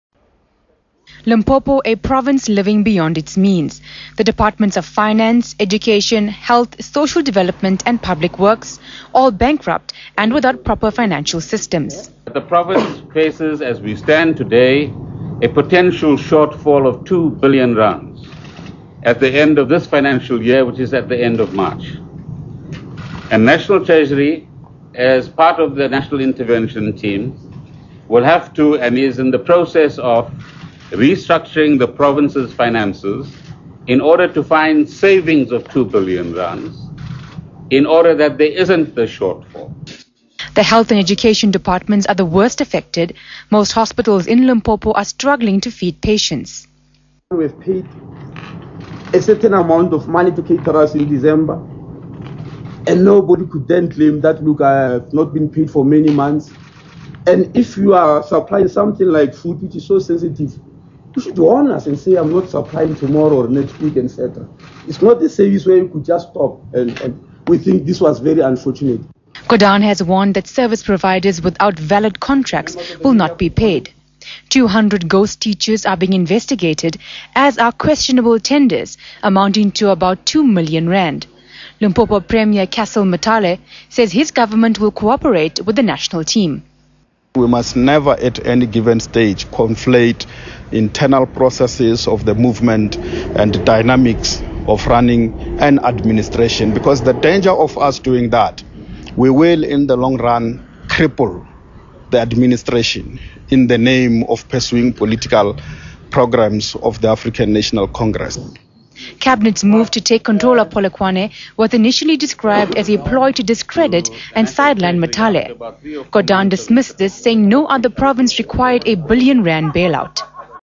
Bankrupt Limpopo placed under administration
Limpopo Premier Cassel Mathale say his Government will co-operate fully with the national intervention team. His comments follow Finance Minister Pravin Gordhan's startling disclosures on the Province's financial mess. Dodgy tenders, unverifiable payments, and even ghost teachers have been identified as part of the problem.